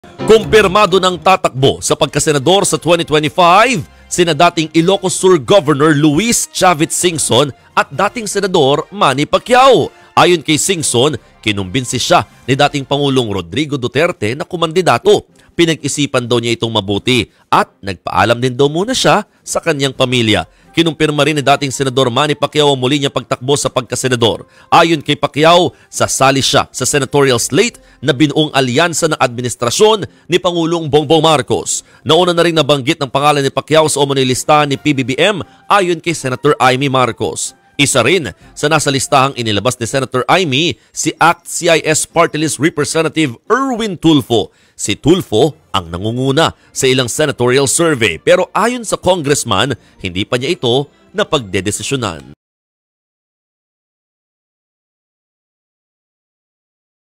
Singson, tatakbo sa pagka-senador sa 2025
Kumpirmado nang tatakbo sa pagka-senador si dating Ilocos Sur governor Chavit Singson. Ayon kay Singson, kinumbinsi siya ni dating pangulong Rodrigo Duterte na tumakbo sa 2025 midterm elections.